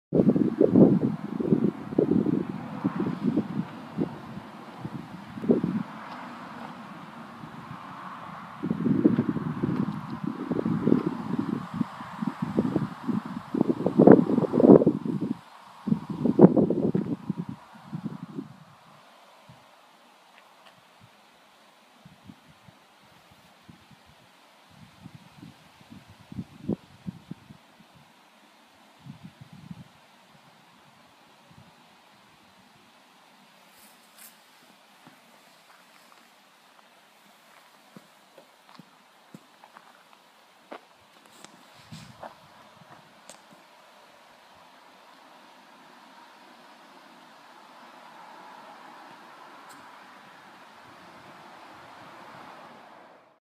thornton abbey
Thornton Abbey and gatehouse near Immingham, Lincolnshire, now looked after by English Heritage